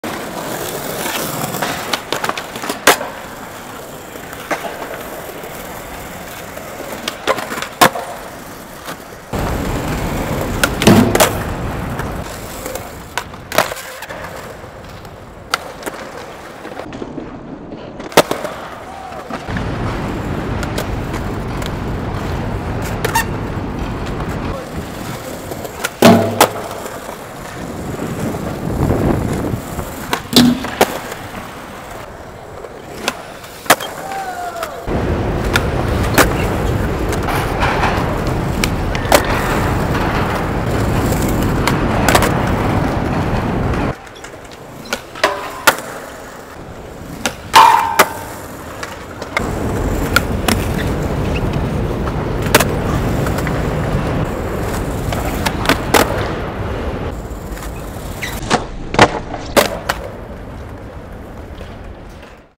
Full Clip Friday : Richard Carrera
Full Clip Friday!
Filmier: Javi Javwad Lopez
Skater: Richard Carrera Stance: Goofy  Age: 14 [ Turned 14 today 3.15.13 :) ]